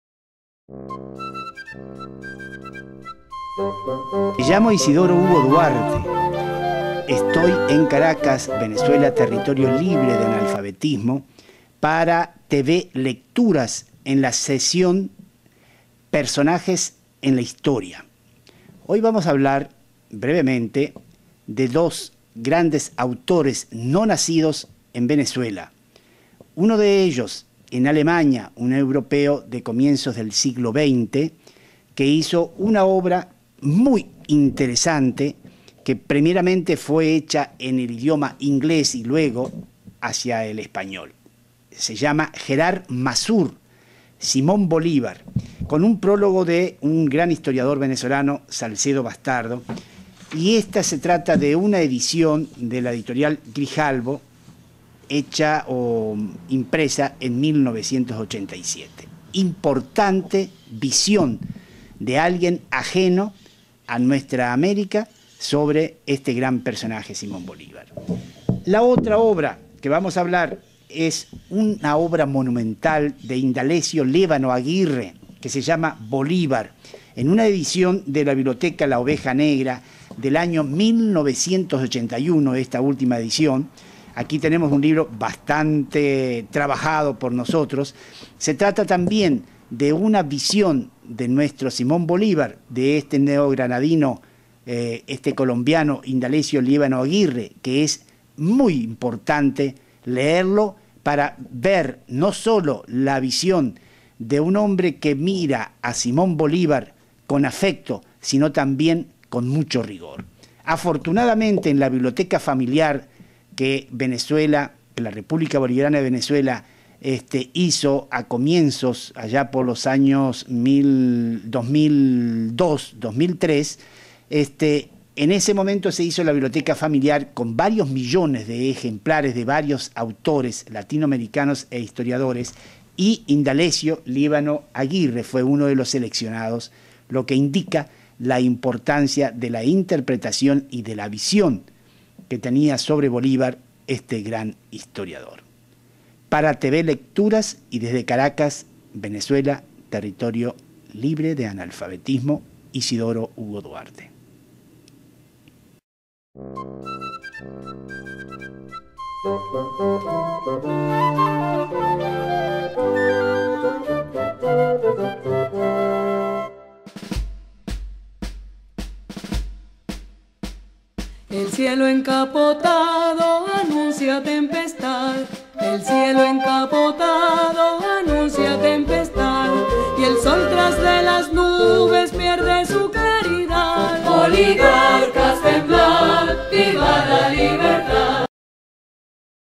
Personajes de la historia en los libros#2 Las mejores biografías de Simón Bolívar para
Personajes de la historia en los libros#2, es un micro video de Isidoro H.Duarte para en el que expresa , a su juicio, cuáles son las mejores biografías de Simón Bolívar

TVLecturas: Isidoro H.Duarte